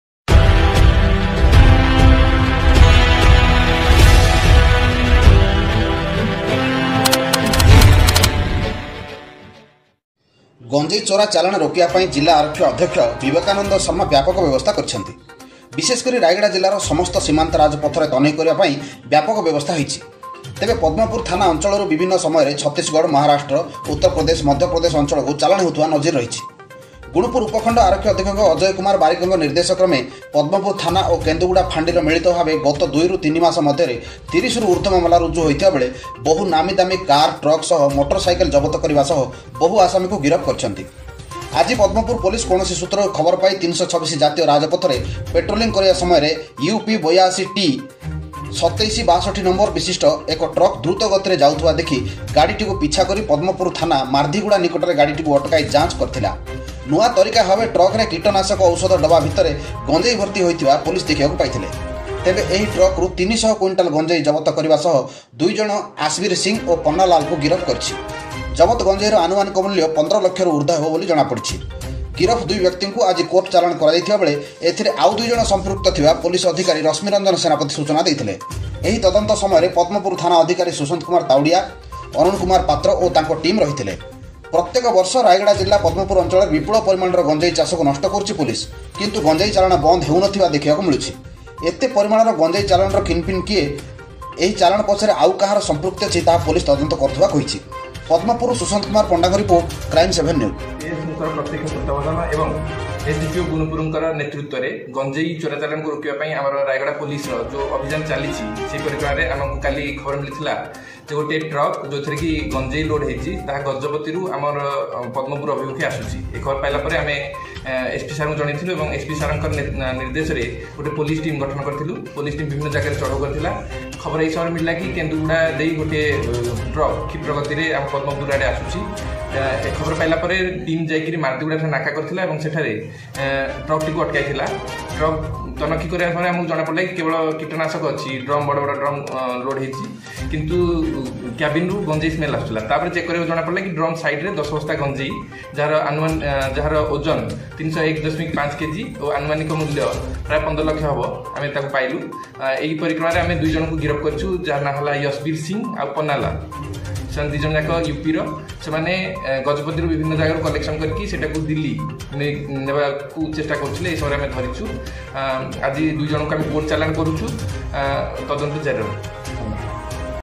ପଦ୍ମପୁର:-ନୁଆଁତରିକାରେ ଗଞ୍ଜେଇ ଚୋରାଚାଲାଣ ୩୦୦କେଜିଗଞ୍ଜେଇ ସହ ଟ୍ରକ ଓ ଦୁଇଗିରଫ
Video from मेरे बालाजी मेरी सरकार